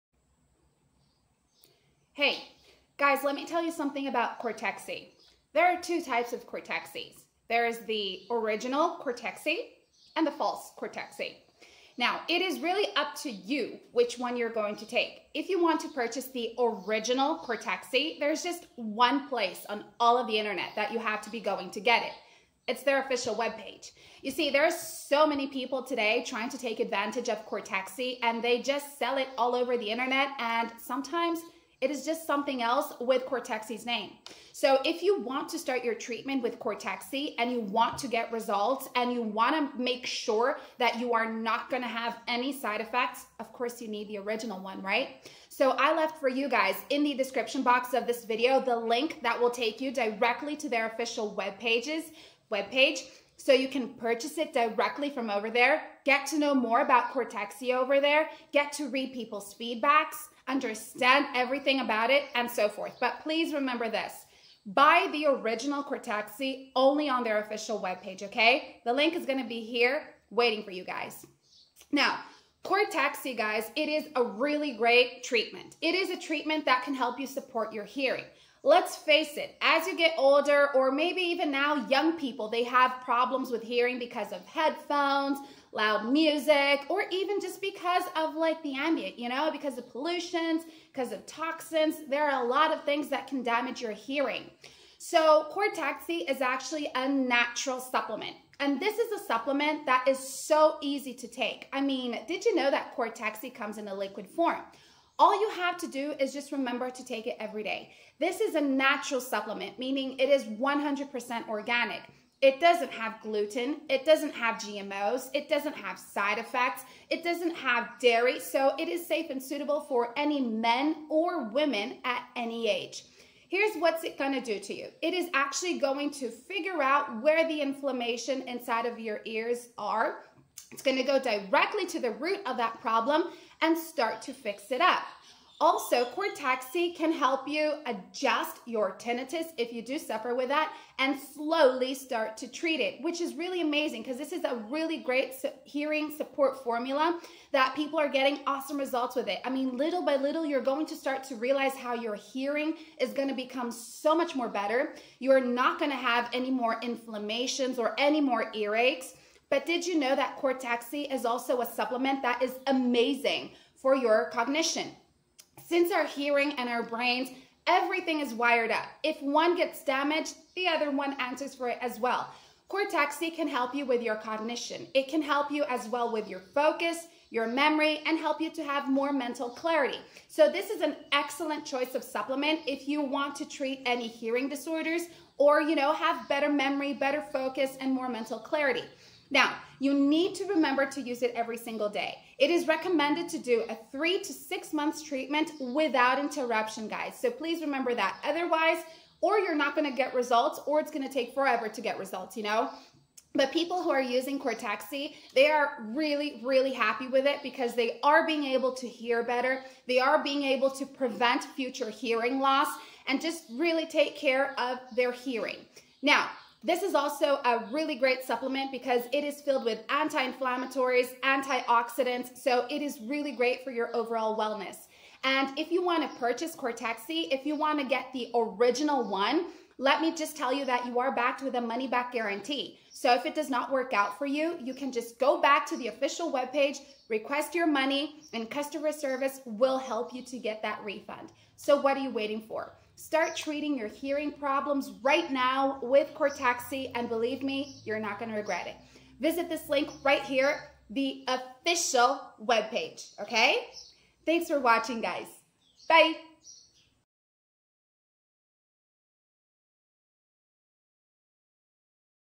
ZENCORTEX - ZEN CORTEX REVIEW [⚠️BEWARE !!] ZEN CORTEX SUPPLEMENT REVIEWS - HEARING SUPPORT
Zencortex is a natural supplement designed to support healthy hearing using a blend of plant-based ingredients. With just one full dropper of Zen cortexs liquid formula taken daily, you can benefit from its unique combination of maca, grape, and guarana, among other natural compounds, to enhance your hearing function.
But does Zencortex really help with hearing loss, and how does it work? In this review, we'll delve into the details of Zencortex's hearing support formula to provide you with all the information you need.

✅ WHAT IS ZEN CORTEX? Zencortex is a dietary supplement formulated to offer comprehensive support for your hearing. Exclusively available online through its official website, this supplement is crafted from natural ingredients to promote overall ear health and address various auditory concerns. Whether you're experiencing hearing loss or simply aiming to maintain optimal ear health, Zencortex may offer the relief you seek.
✅ BENEFITS OF ZENCORTEX According to the official website, Zencortex offers several potential benefits, including:
A 100% natural formula comprising plant-based ingredients
Support for healthy hearing function
Enhancement of mental sharpness, memory, and cognitive abilities
Reduction of external noise and interference
Improved clarity in hearing loved ones and your favorite music
Minimal to no side effects
Easy incorporation into your daily routine by adding it to your coffee or beverage of choice. Dosage recommendations vary, with some users taking one or two drops daily and others opting for a full dropper. The flavorless formula works quickly, with many customers reporting noticeable effects within a week.

✅ HOW DOES ZENCORTEX WORK? Zencortex functions through four key mechanisms:
Supporting Healthy Hearing: Zencortex contains natural, plant-based ingredients aimed at promoting the health of your ear's hair cells, which are responsible for processing external noise and transmitting signals to your brain. By maintaining the integrity of these cells, Zencortex supports healthy hearing function.

Enhancing 360-Degree Hearing: Zencortex may improve your ability to hear sounds from all directions, facilitating better auditory processing and overall ear health.
Strengthening Memory: The supplement contains ingredients that can bolster memory function, ensuring that your brain receives accurate auditory signals and facilitating optimal memory formation.
Sharpening Mental Acuity: Zencortex promotes mental clarity, enabling your brain to function more effectively, particularly when auditory distractions are minimized.

✅ WHO SHOULD CONSIDER TAKING ZENCORTEX? Zencortex is specifically formulated for individuals in their 30s, 40s, 50s, 70s, and beyond who are concerned about their hearing and ear health. Whether you're grappling with tinnitus, struggling to sleep due to persistent ear noises, or experiencing symptoms of hearing loss, Zencortex may offer valuable support. Additionally, individuals exposed to frequent loud noises or seeking long-term protection against hearing loss may also benefit from Zencortex's supplementation.

✅ WHERE TO BUY ZENCORTEX? Zencortex is exclusively available for purchase on its official website.

✅KEY MOMENTS:
00:00 - Introduction Zencortex
00:35 - Importance of purchasing the original ZenCortex on the official website to avoid counterfeit products
01:05 - Description of ZenCortex as a treatment to support auditory health
01:39 - Discussion about the prevalence of hearing problems and the effectiveness of ZenCortex
02:11-  Description of ZenCortex  as a liquid natural supplement for easy ingestion
02:42 - Benefits of ZenCortex for auditory health, including treatment of inflammation and ringing
03:15 - Benefits of ZenCortex  for cognition, memory, focus, and mental clarity
03:46 -  Recommendation for consistent daily use of ZenCortex  for optimal results